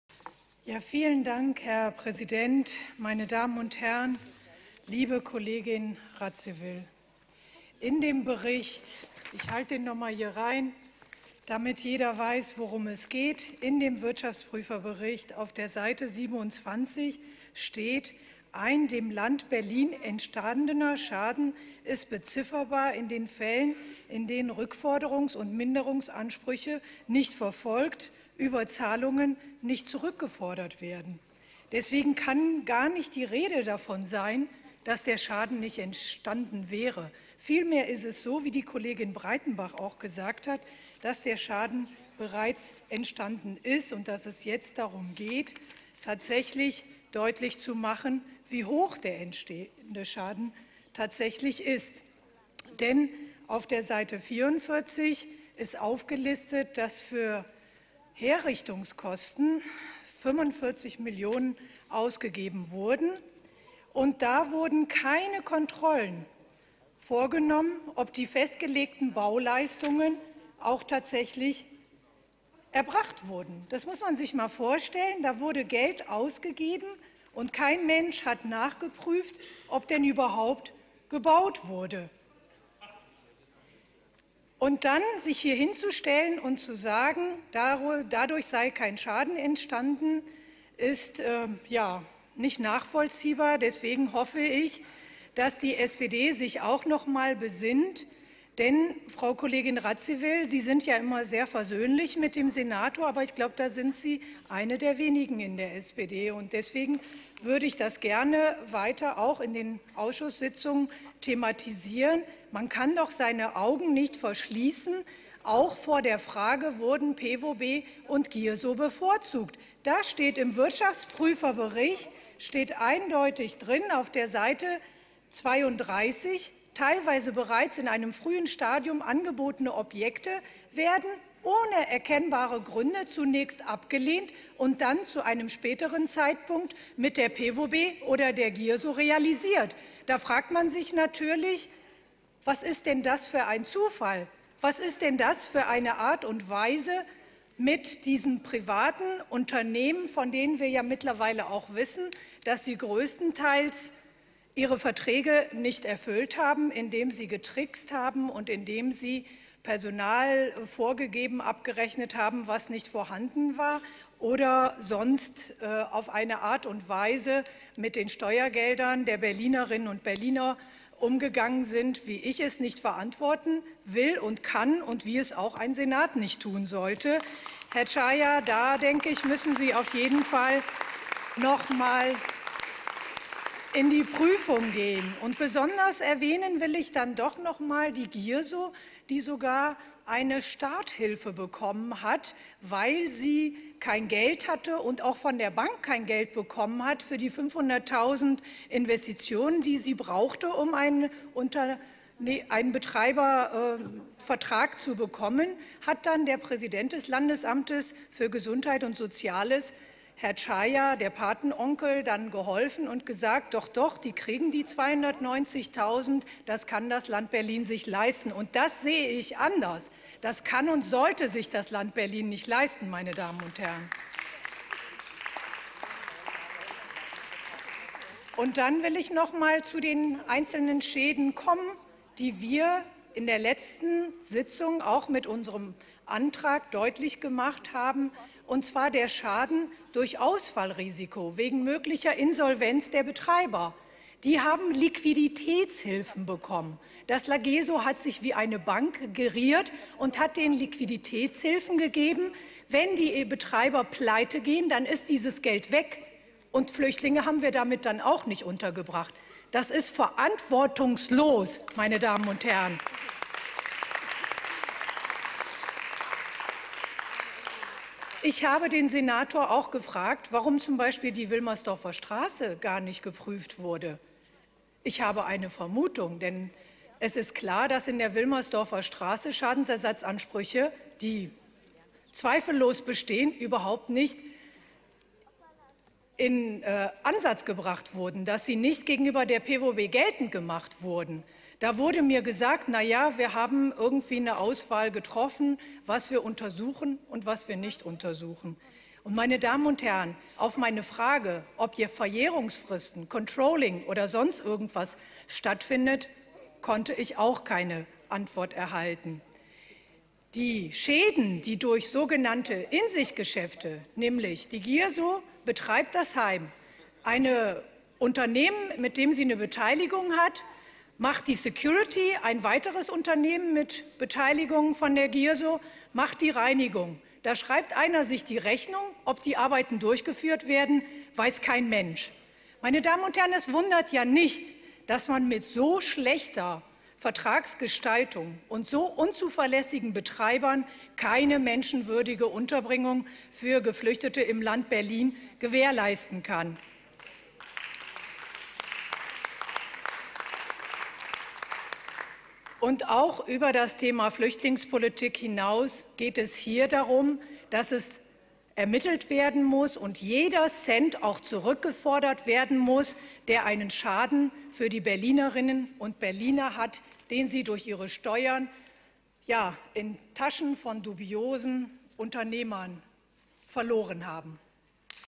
Canan Bayram zu Vergabepraxis des Landesamtes für Gesundheit und Soziales (LaGeSo)
Canan Bayram (Bündnis 90/Die Grünen) kritisiert, dass es gar nicht darum gehe, ob überhaupt ein Schaden für das Land Berlin bei der Flüchtlingsunterbringung entstanden sei. Vielmehr gehe es laut Wirtschaftsprüfer-Bericht nur noch um die Höhe des Schadens. So seien 44 Millionen Euro Herrichtungskosten ohne Nachweis über eine tatsächliche Erbringung der Bauleistungen abgerechnet worden. Außerdem seien bei der Vergabe von Flüchtlingsheimen zwei umstrittene Betreiber bevorzugt worden. Andere Bewerber seien immer wieder ohne Gründe abgelehnt worden. Sie hoffe, dass sich die SPD "auch noch mal besinnt", da man nicht einfach sagen könne, es sei kein Schaden entstanden.[ Quelle